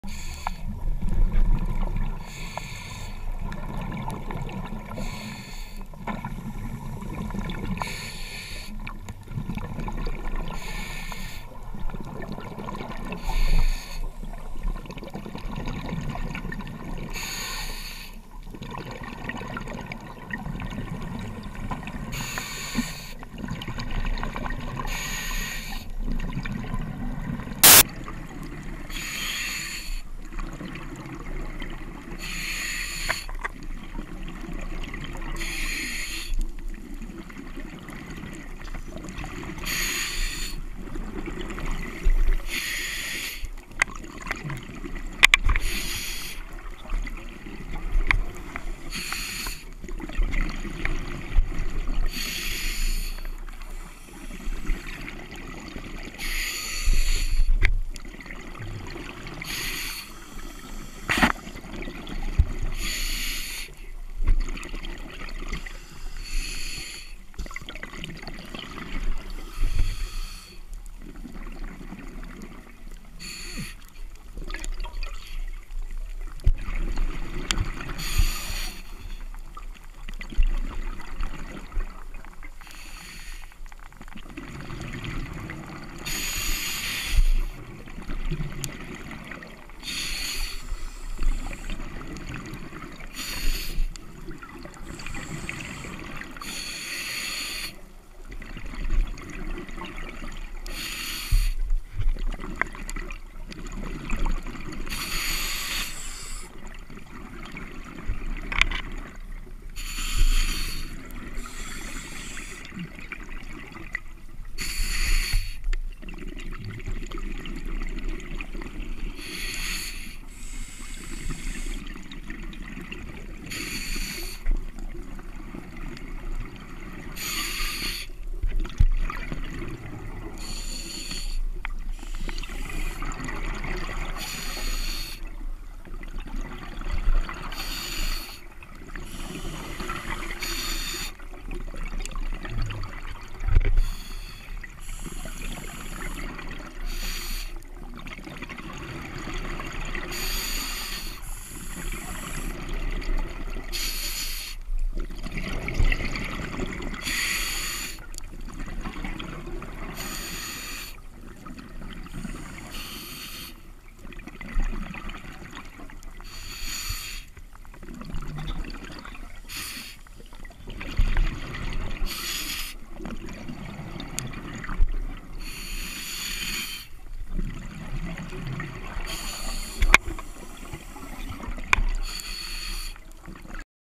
The Pinnacle- Fiji 2013
Gopro Hero 3 silver addition. Spotted eagle ray, swim through and lots of fish with decent visibility in 25+ metres of water